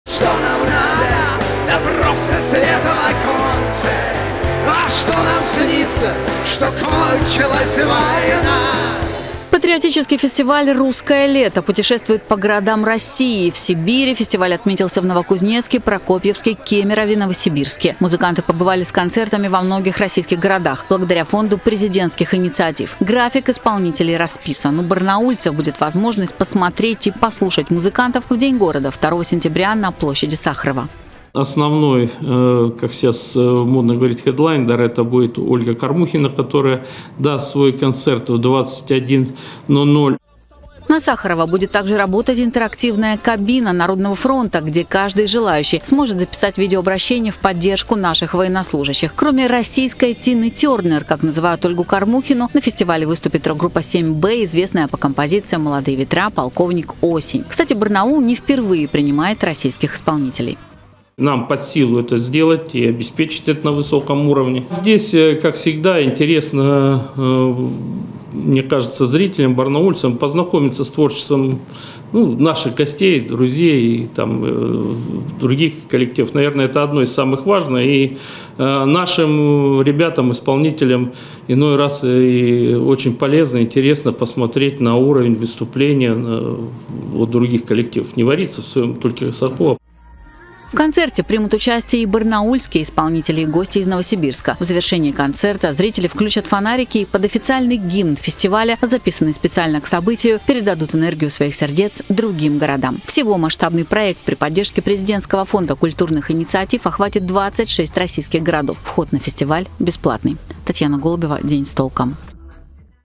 В Барнауле в День города пройдет патриотический фестиваль "Русское лето. За Россию"
В фестивале примут участие российские музыканты. Пройдет он на площади Сахарова.